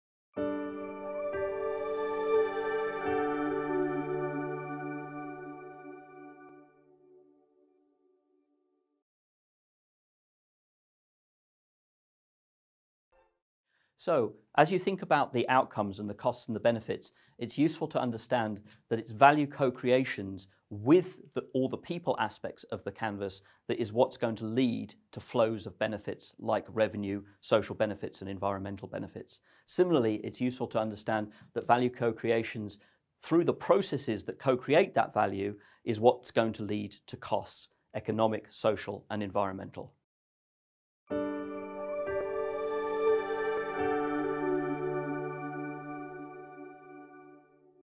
Key Flows and Interconnections (4/5)
Forth of five videos introducing the Flourishing Business Canvas.

This video was created by Antony Upward / Edward James Consulting with leading First Explorers of the Flourishing Business Canvas Lean for Flourishing Startups and the Pan-EU Climate Change initiative EIT-Climate-KIC

FBC104